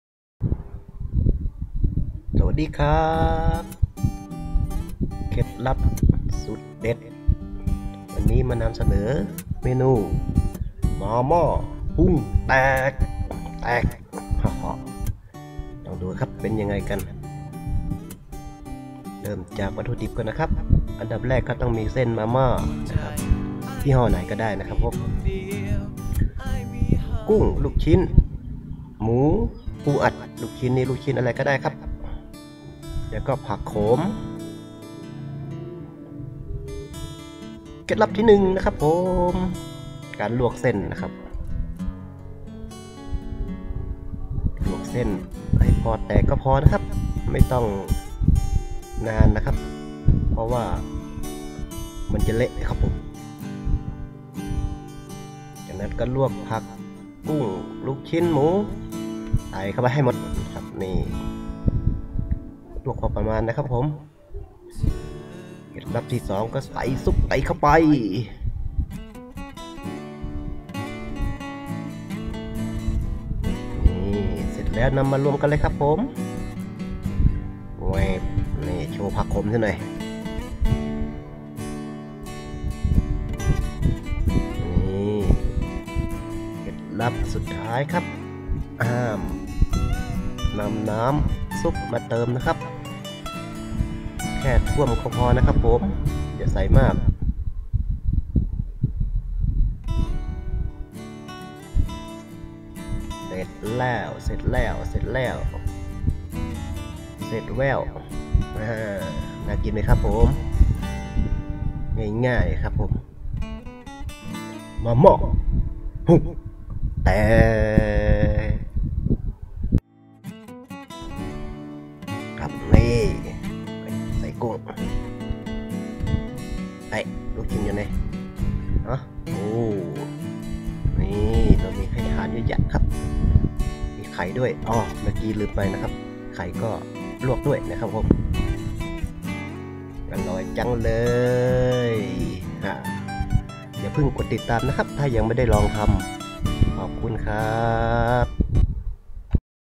มาม่าพุงแตก@ 2 นาที#
มาม่าพุงแตก ภายใน 2 นาที เมนูง่ายๆ ใครก็ทำได้  กับ สูตรเด็ดเคล็ดลับกับ อ้ายปั๋น